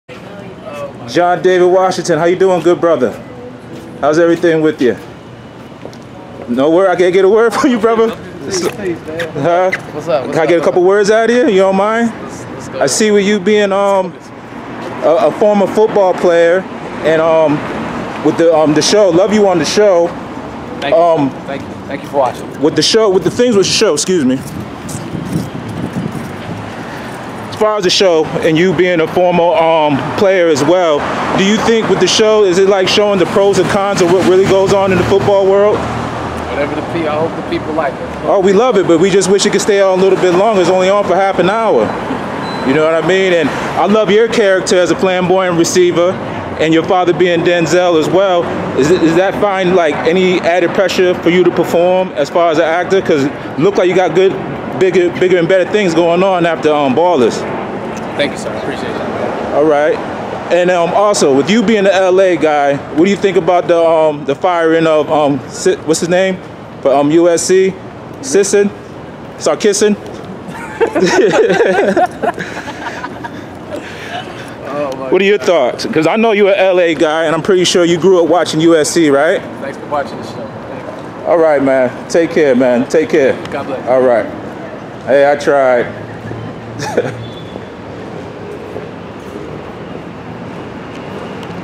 Denzel’s Son John David Washington of HBO’s Ballers Strolls Like Ricky Jerret in Beverly Hills
Former professional football player turned actor, John David Washington, is spotted while out and about in Beverly Hills, CA with a lady friend.

Washington who now stars on the hit HBO series "Ballers" as Ricky Jerret resisted all attempts to ask him questions but was gracious enough to thank us for liking the show.

If he looks familiar, maybe it's because his dad is 2 time Academy Award winner Denzel Washington.